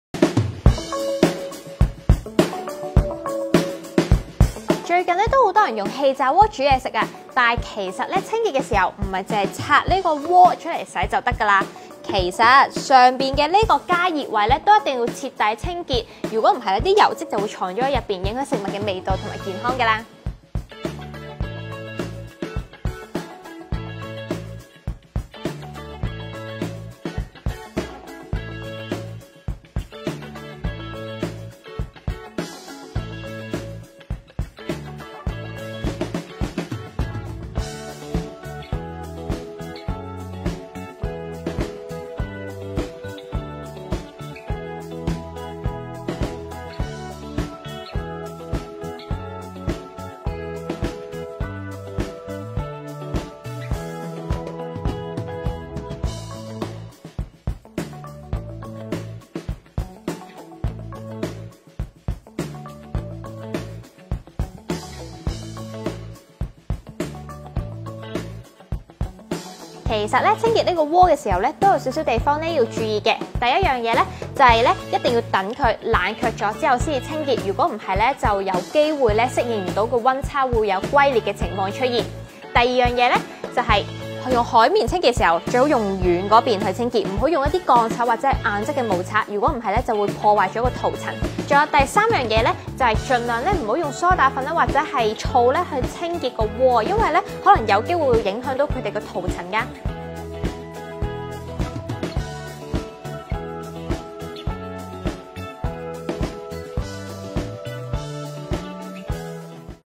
實測！清潔氣炸鍋「蚊香區」方法+清潔內鍋小Tips
最近好多人都用氣炸鍋，其實清潔氣炸鍋嘅「蚊香區」位置係非常重要！小編就見到坊間有方法教大家點清潔，即刻幫大家實測下得唔得先！

試完之後，小編嘅個人建議就係記得每次用完都要用布抹「蚊香區」位置，否則油污長期積聚變萬年油污，就好難好難先洗得甩㗎啦😢

**以上實測的坊間清潔方法只供參考，市面上氣炸鍋有大量不同款式，最好向所屬廠商諮詢清潔方法

全方位緊貼《U Magazine》社交平台